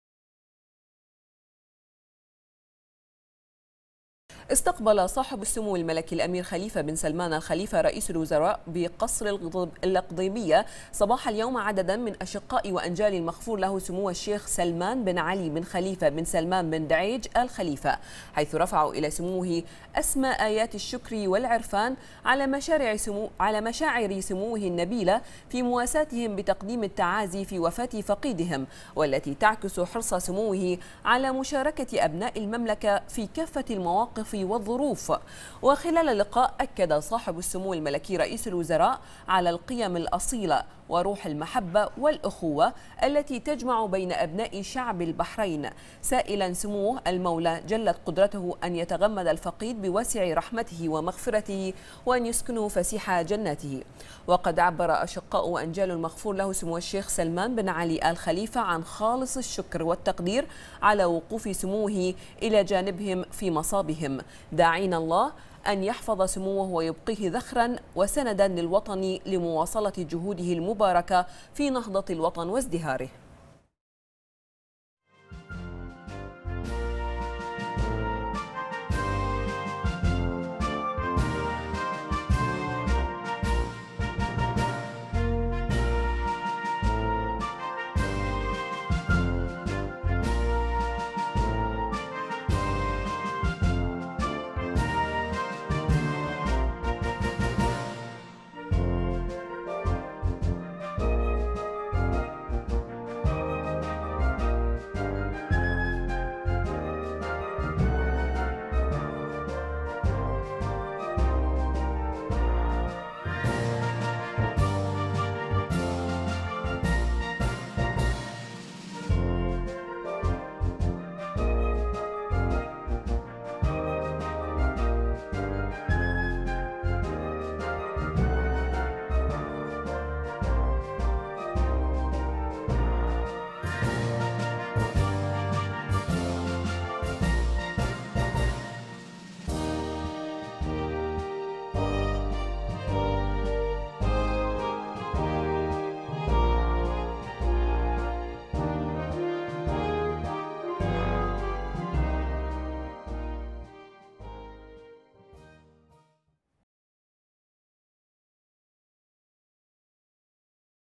البحرين: سمو رئيس الوزراء يستقبل عدداً من أشقاء وأنجال المغفور له سمو الشيخ سلمان بن علي بن خليفة
المنامة في 29 أغسطس / بنا / استقبل صاحب السمو الملكي الأمير خليفة بن سلمان آل خليفة رئيس الوزراء بقصر القضيبية صباح اليوم عددا من أشقاء وأنجال المغفور له سمو الشيخ سلمان بن علي بن خليفة بن سلمان بن دعيج آل خليفة، حيث رفعوا إلى سموه أسمى آيات الشكر والعرفان على مشاعر سموه النبيلة في مواساتهم بتقديم التعازي في وفاة فقيدهم، والتي تعكس حرص سموه على مشاركة أبناء المملكة في كافة المواقف والظروف.